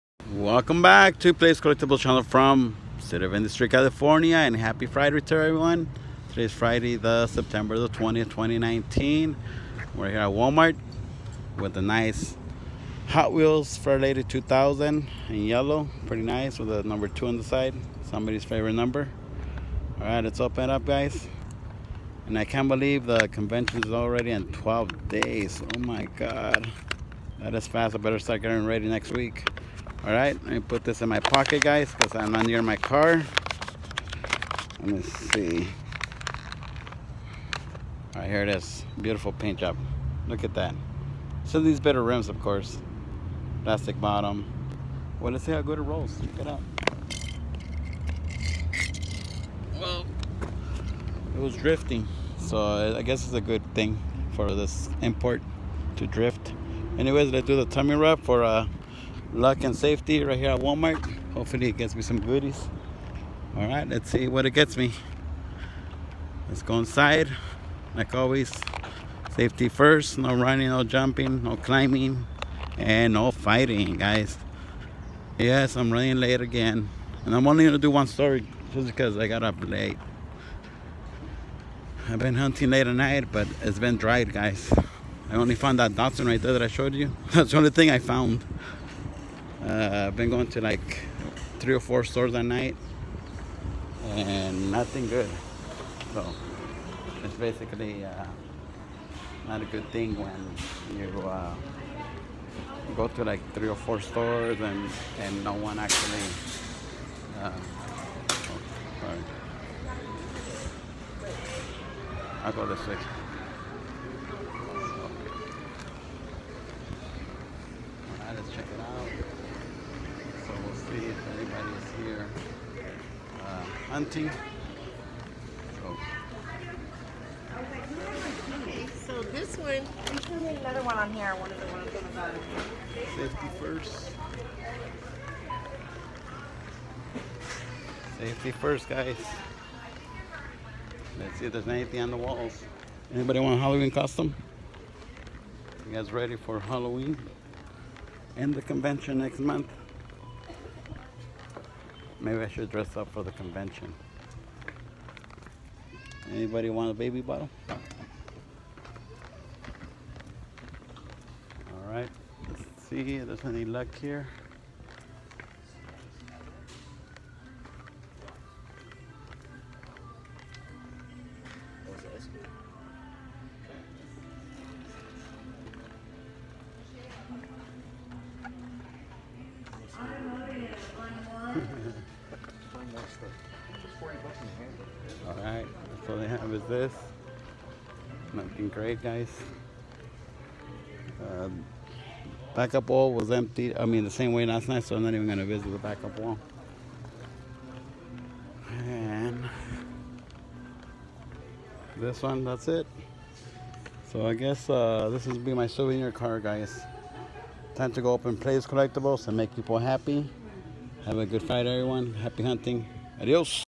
Playdays Collectibles Friday morning Hotwheels hunting at Walmart!. 9.20.19
Good morning and happy Hotwheels Friday!. Open 7 days a week. Thank you for your business and support. Don’t forget to visit my YouTube channel